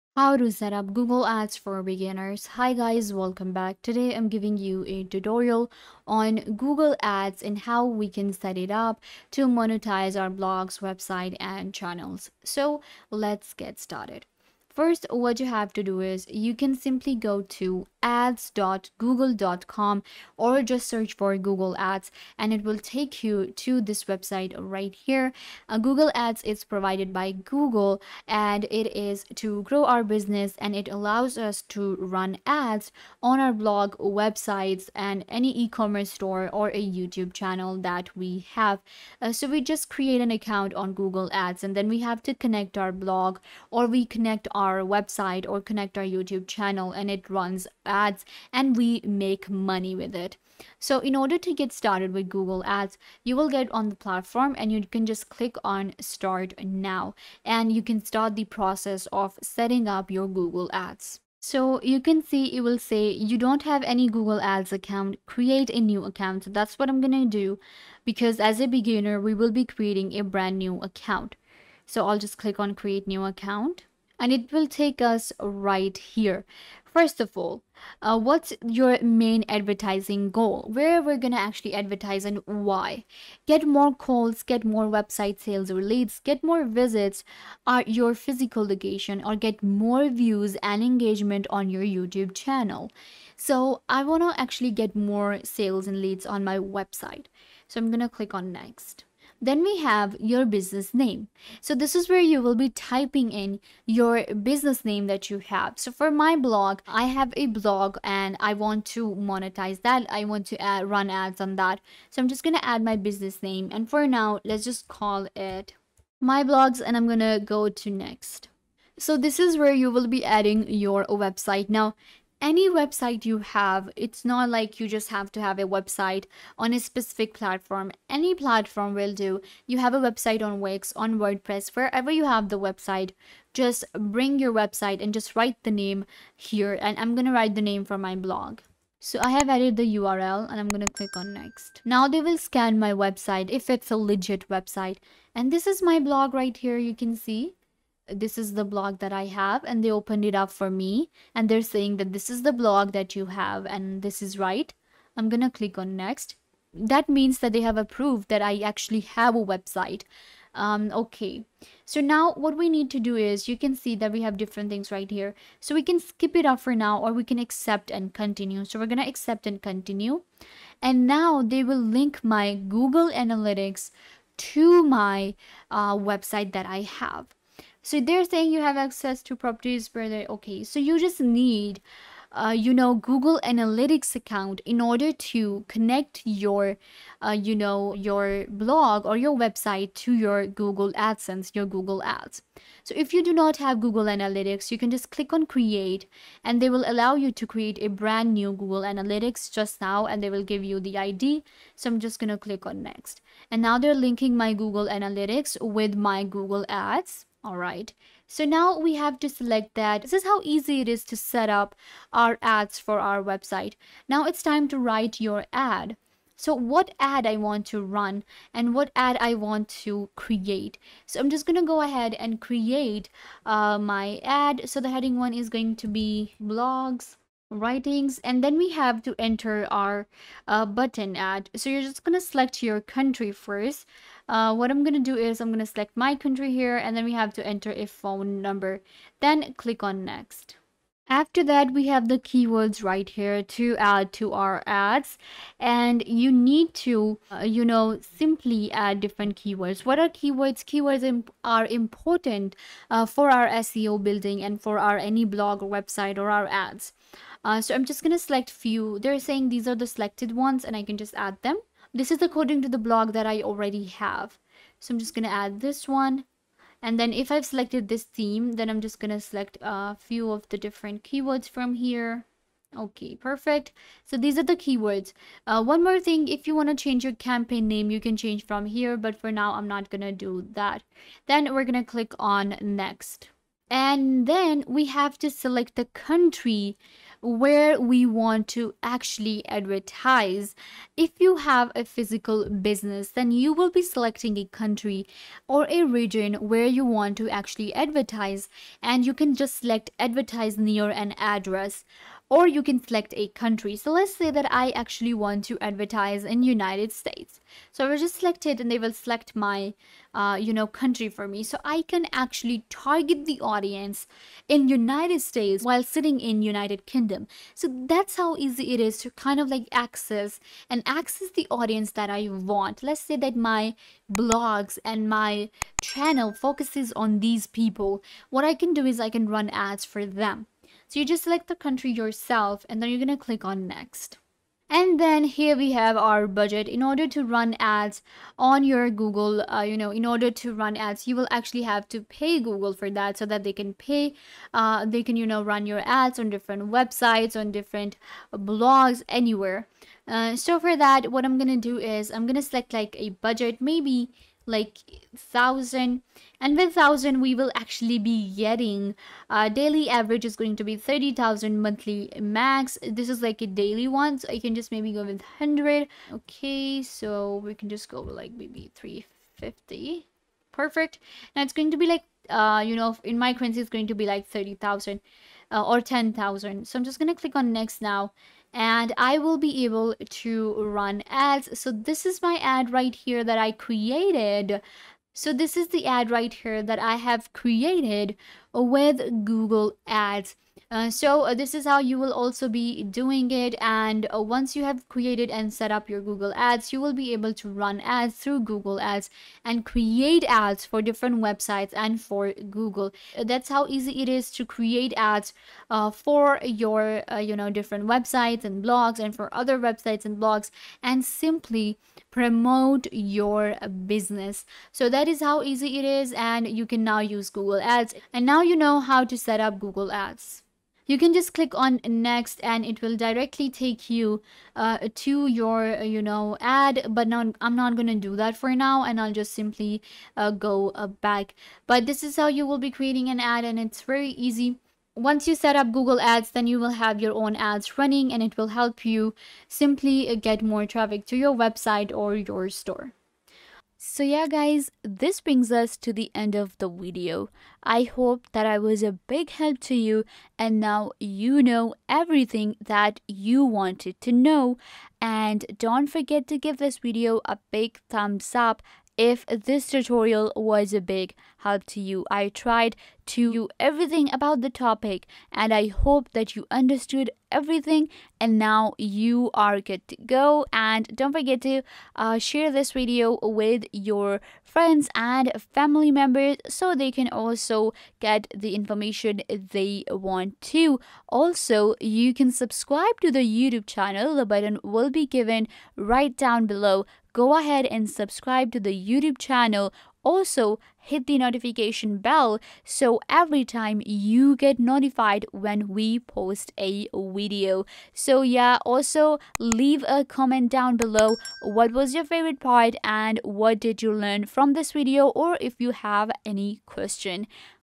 How To Setup Google Ads - For Beginners (2025)
In this video I show you How To Setup Google Ads. This is super easy and learn to do it in just a few minutes by following this helpful tutorial.